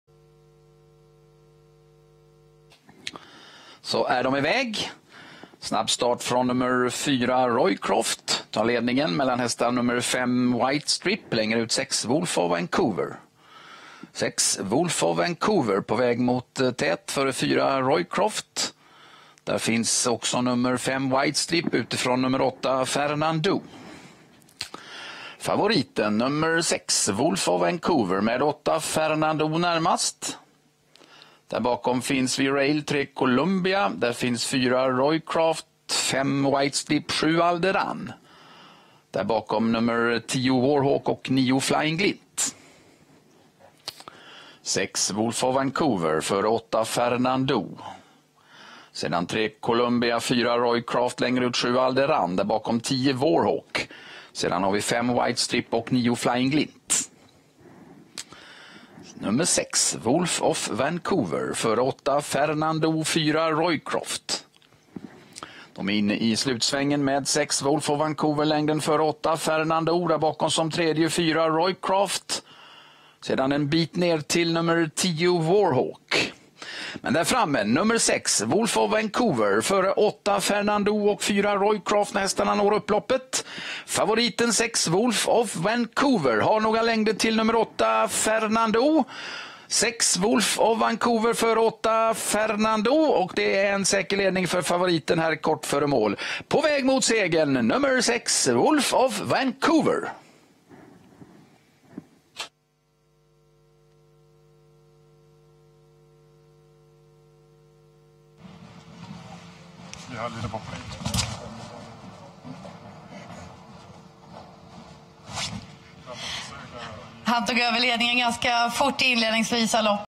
Wolfof Vancouver (CAN) - 11 October 2020
Wolfof Vancouver (CAN)
1st at Jägersro, 11 October 2020
Owner: The JC Organization
Trainer: Lennart J:r Reuterskiöld
Jockey: Jacob Johansen

A €130,000 purchase by Filip Zwicky Agency at Goffs Orby Sale 2019.